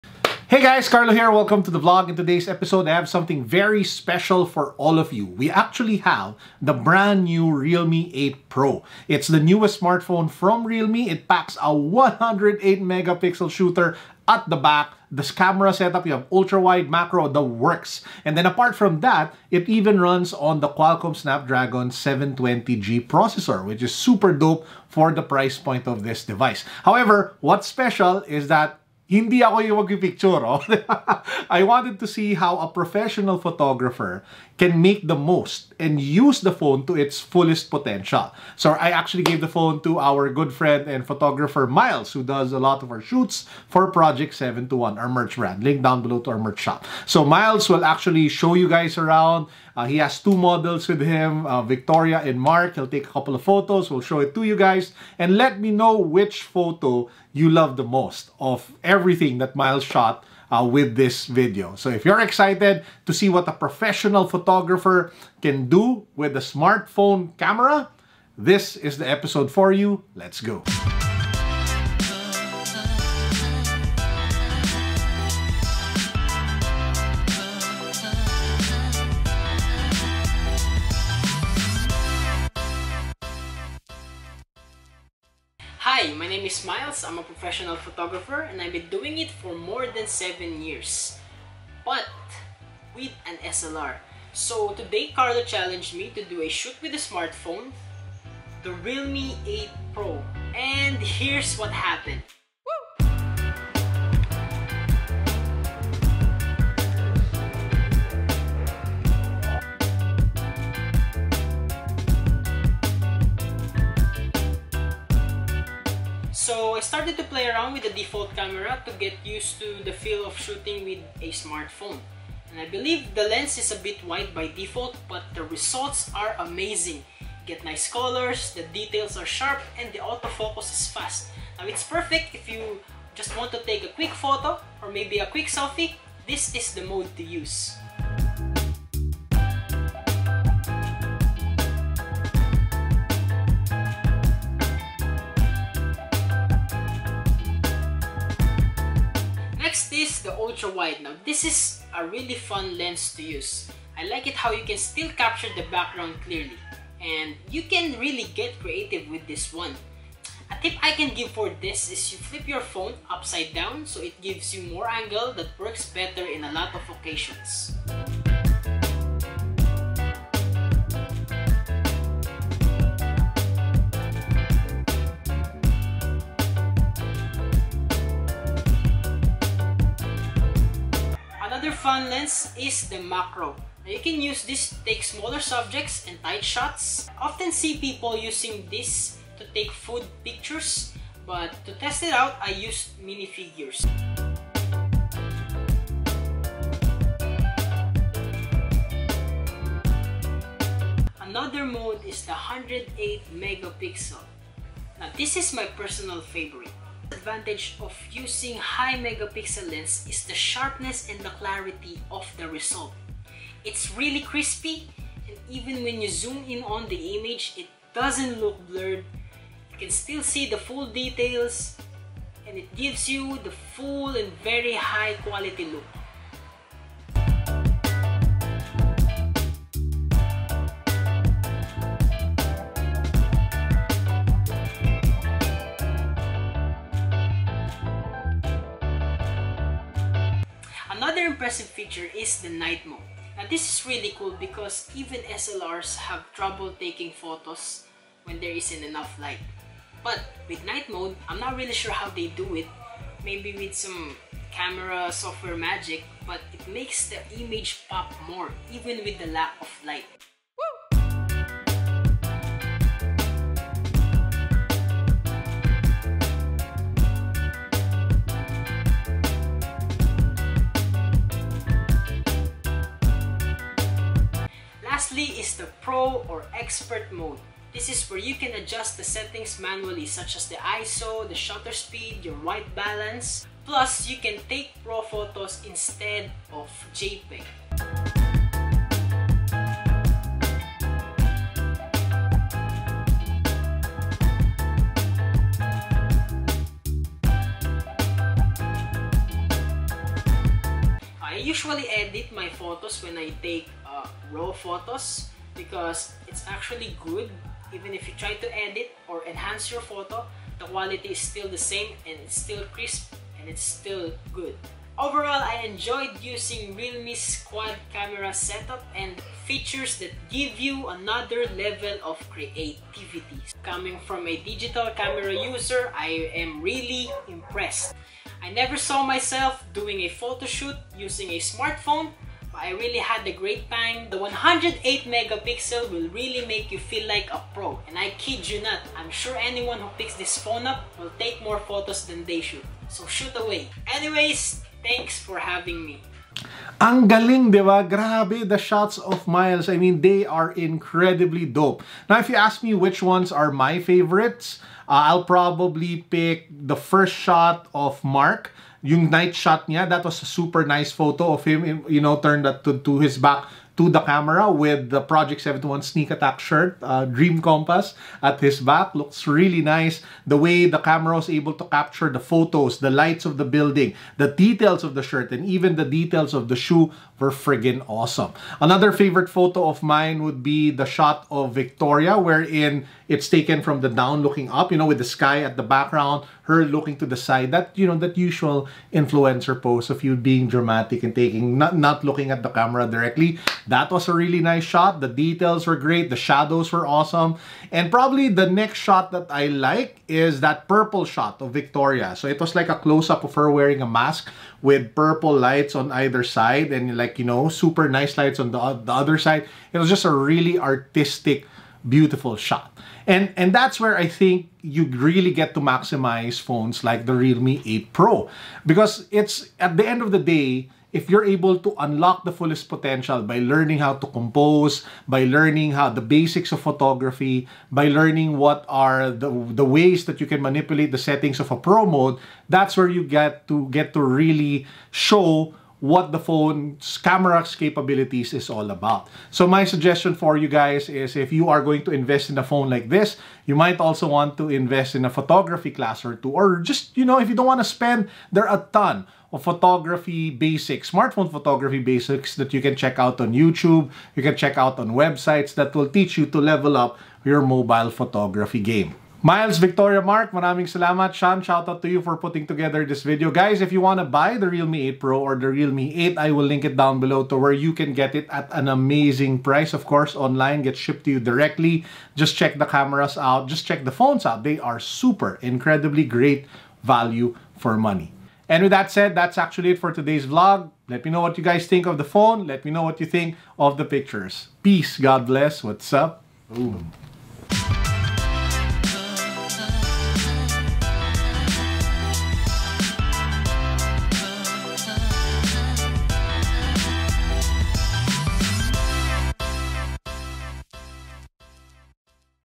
We CHALLENGED a Pro Photographer to Shoot with a Smartphone (RESULTS)
In today's episode we have something special. We challenged a professional photographer to shoot with the realme 8 Pro. It's the newest smartphone from realme that features a friggin 108MP primary camera. Let's find out what he can do with it.